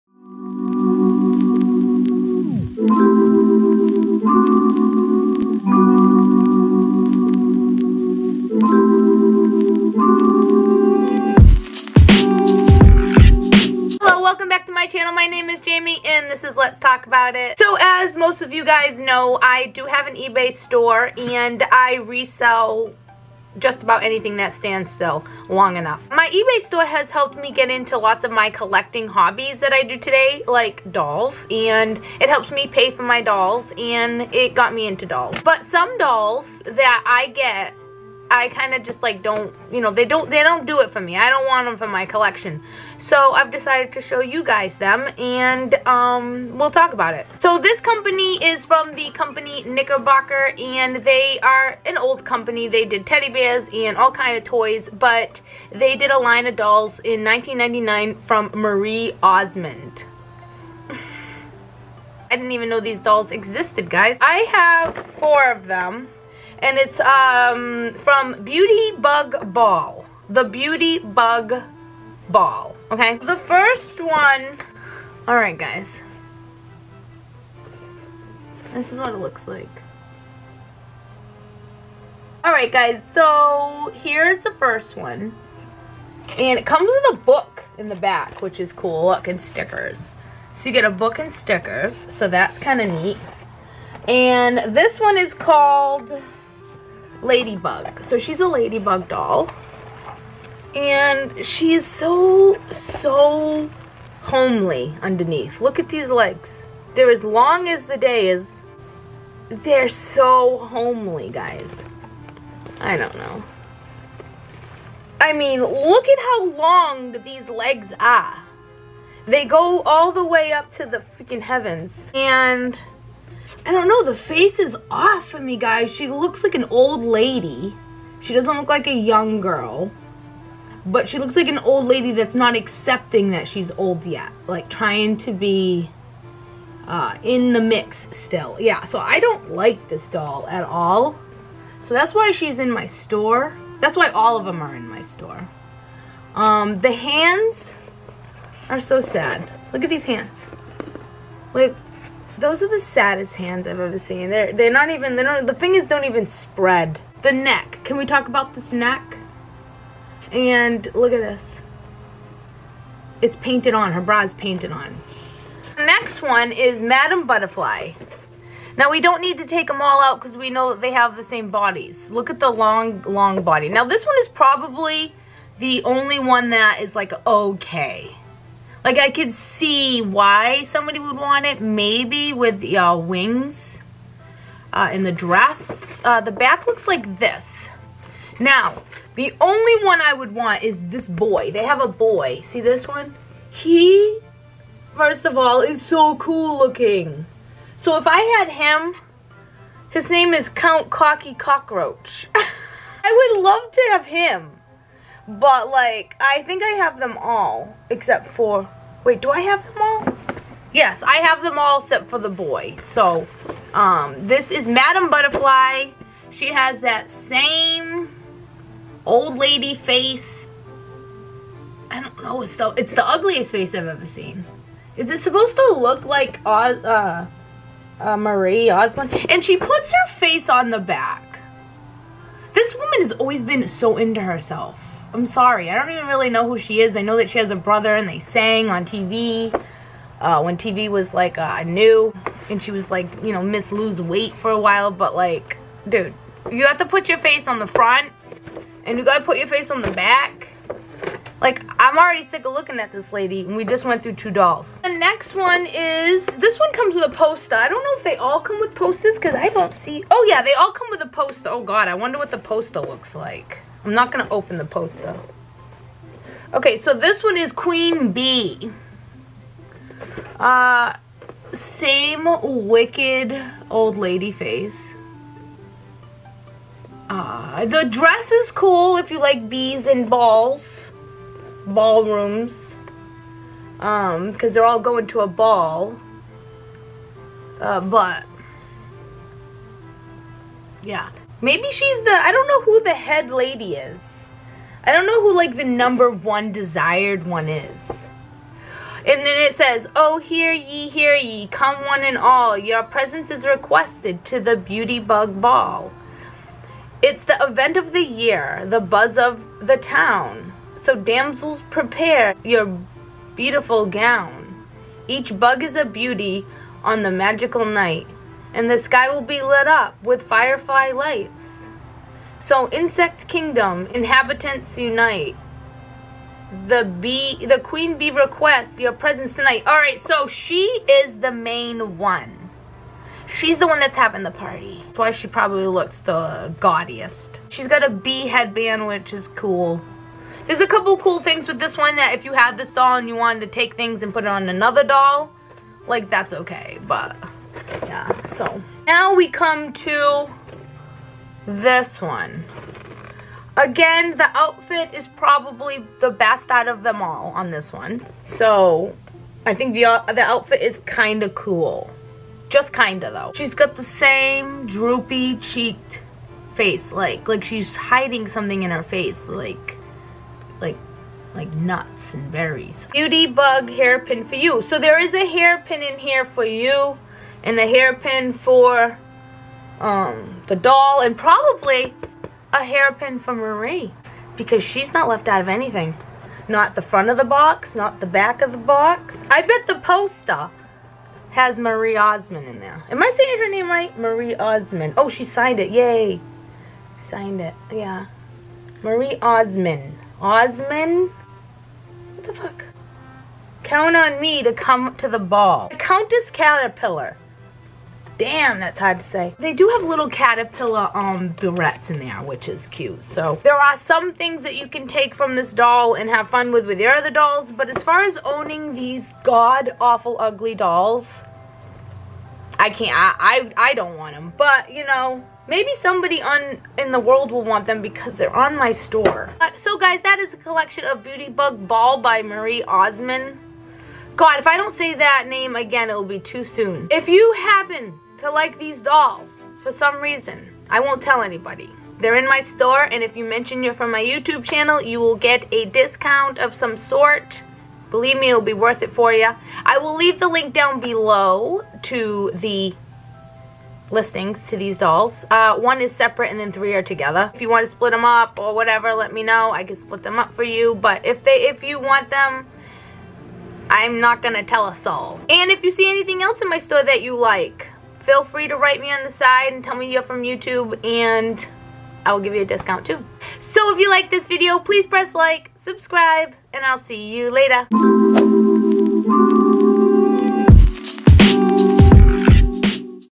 Dolls listed on my eBay shop!The beauty bug ball Marie osmond knickerbocker collection#marieosmond
I’m here with dolls in my shop!todays is marieosmonddolls beautybugball Check them out and if you want them the link is below! Feel free to write me for a discount

Early bird treasures